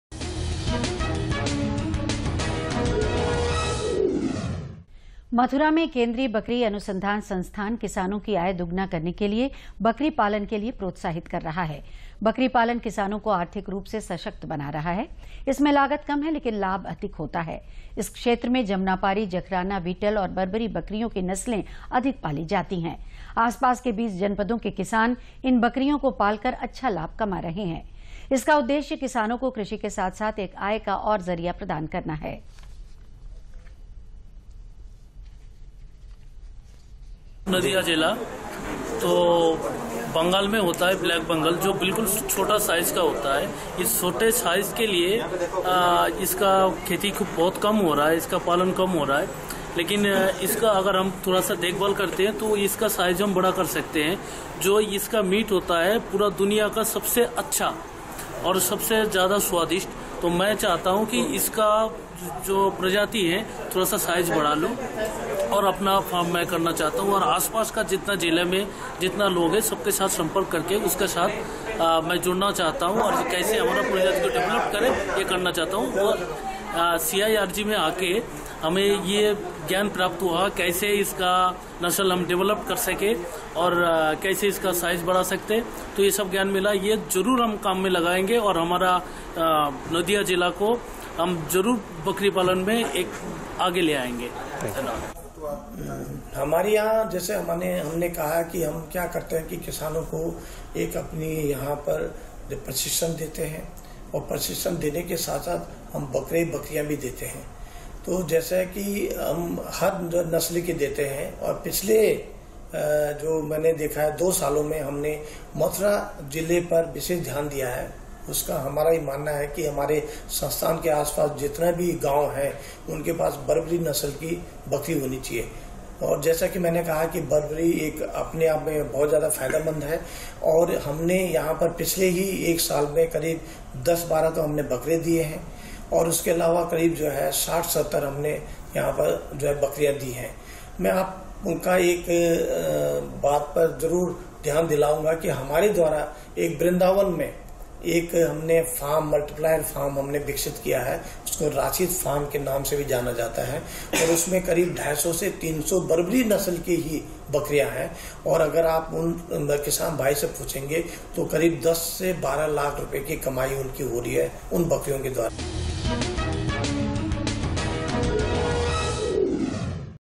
Kisan In Plan- Ground Report from Mathura in UP
बकरी पालन से किसान बढ़ा सकते है अपनी आय